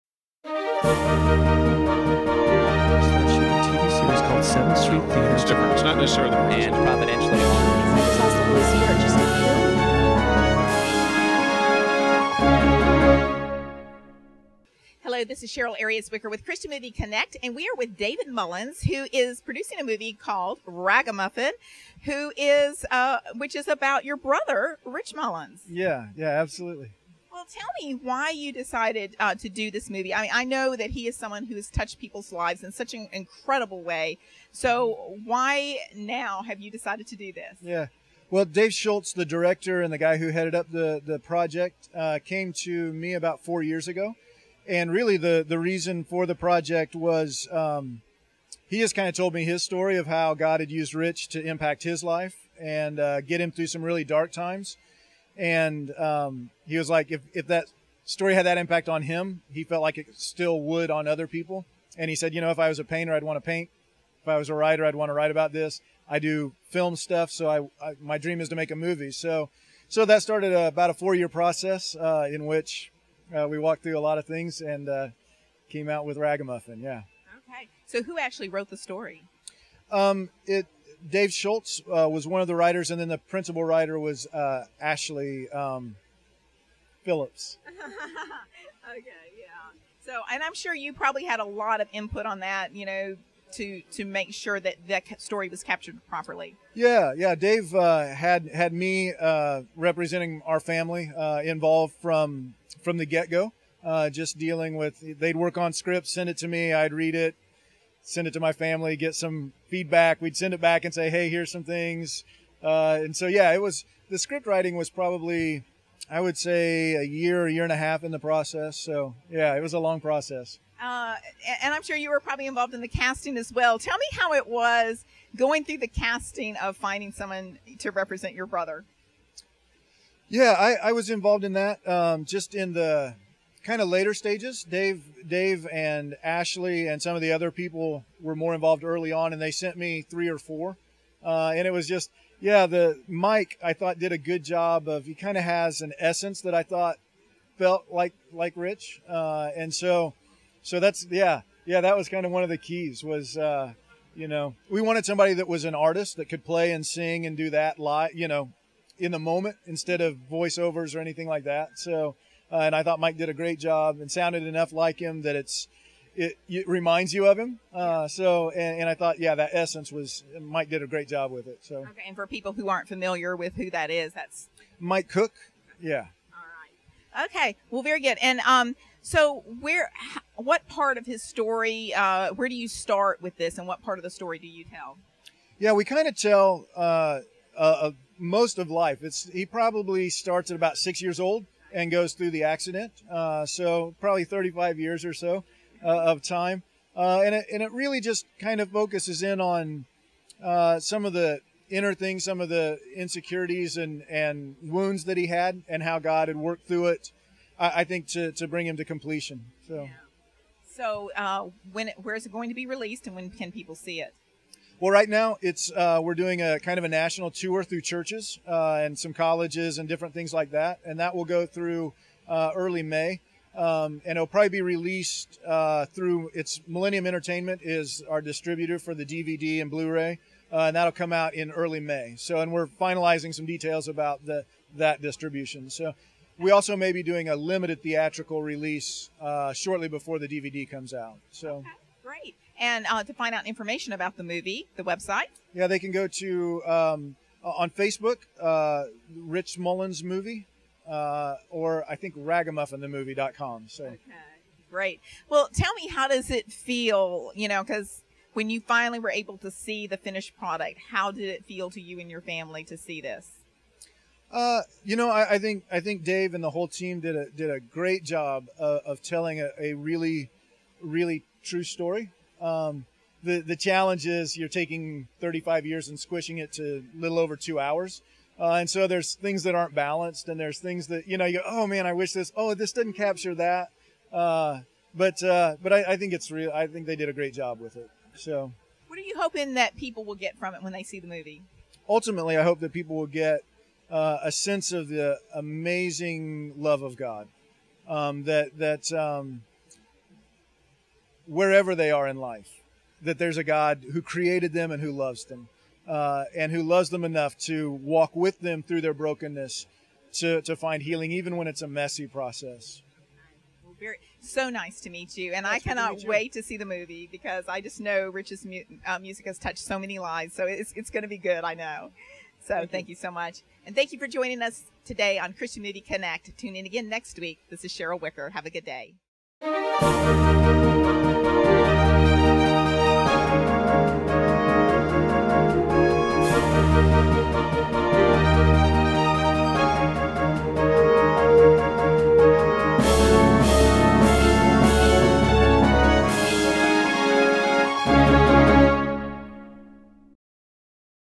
CMC #107 - David Mullins
Cheryl Ariaz Wicker of Christian Movie Connect interviews David Mullins about "Ragamuffin." The film is based on the life of David's brother, Christian musician, Rich Mullins.